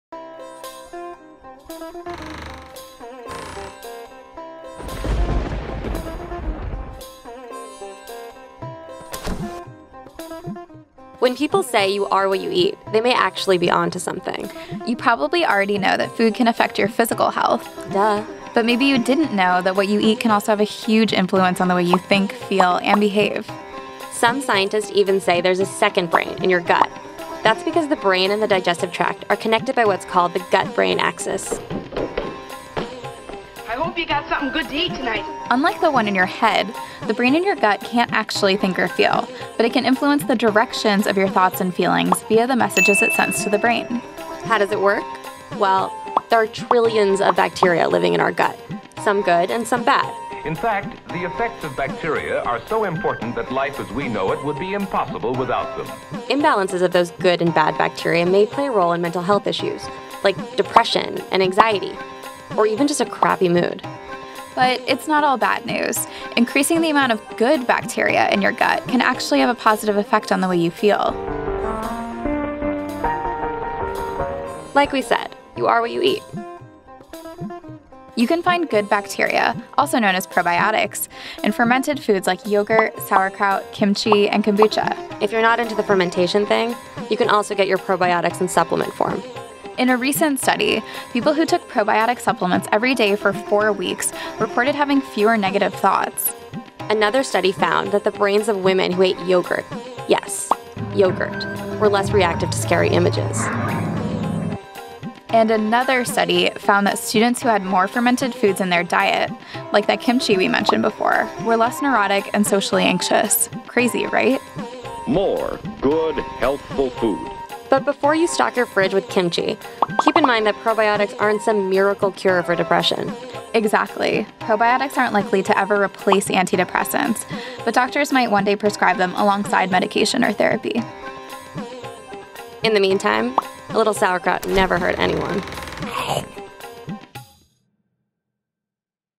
Open Your Mind: Bacteria and Your Mood
A fun, animated science video about probiotics.